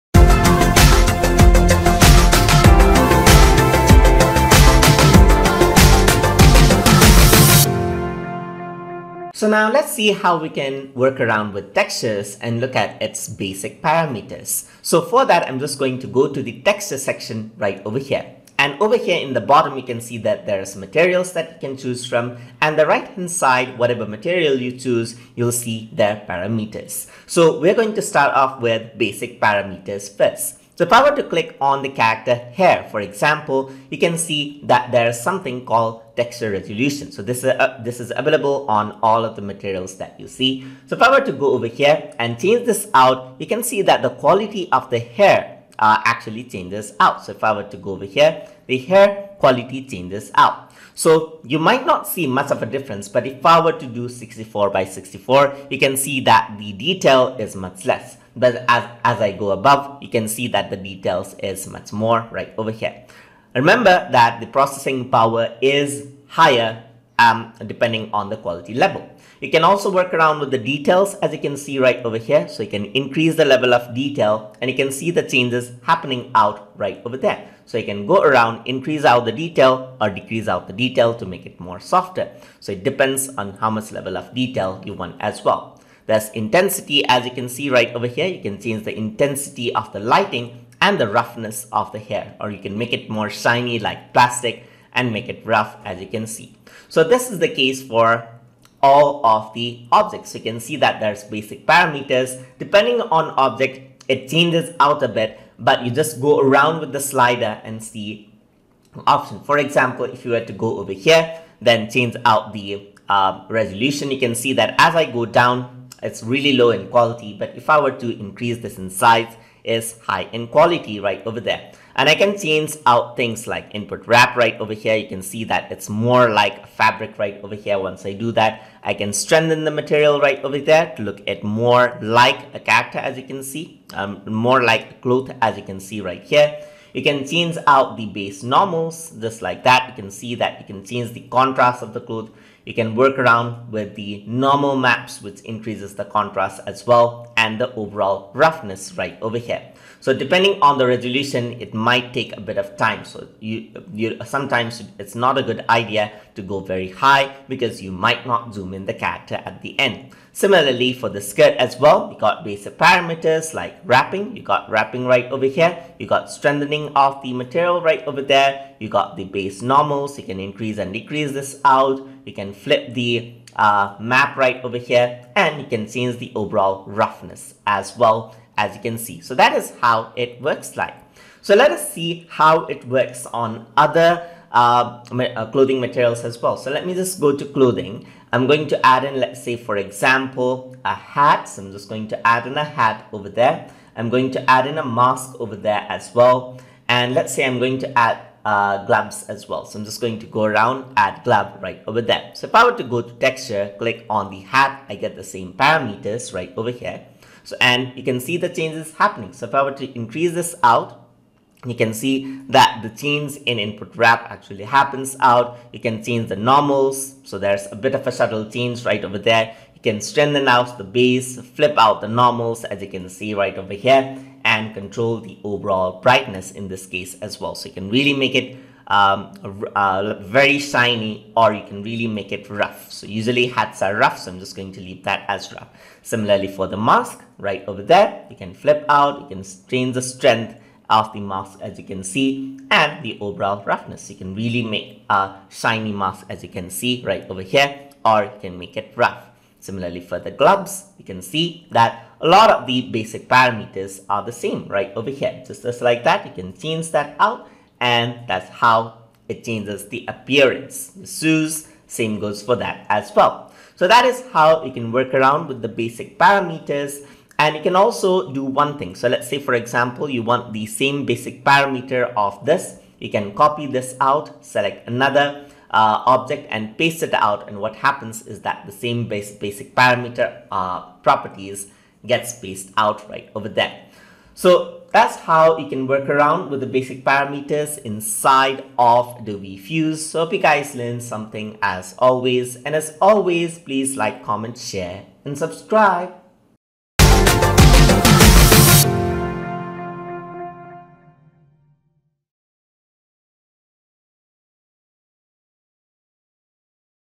Adobe Fuse Tutorial - Lesson 20 - Working with Texture Basic Parameters
In this tutorial, we will be discussing about Working with Texture Basic Parameters in Adobe Fuse

Learn the basics of using Fuse including how to work with head, torso and legs including how to work with textures and exporting the model for different scnarios.

This lesson has been made using Adobe Fuse CC 2017 for Windows but is applicable for newer and older versions and Mac Version of Adobe Fuse Application as well.